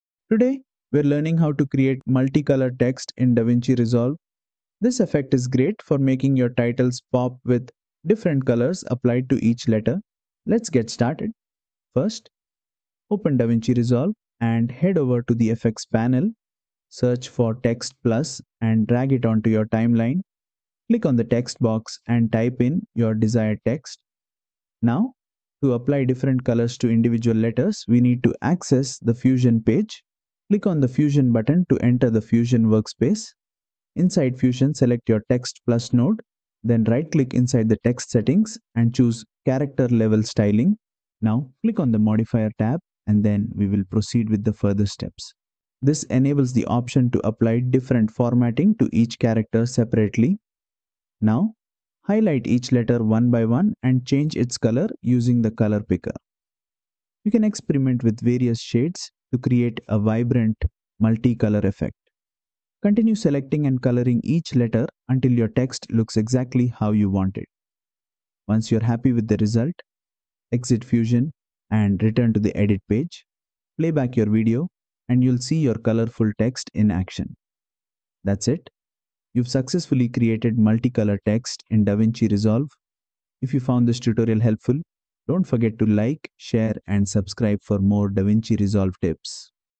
How to create Multi Color Text in DaVinci Resolve - Lesson 140 DaVinci Resolve Tutorial
Want to make your text stand out with multiple colors? In this tutorial, I’ll show you how to apply different colors to individual letters using Character Level Styling in the Fusion Page of DaVinci Resolve. This method is perfect for creating eye-catching titles, creative text animations, or unique typography effects.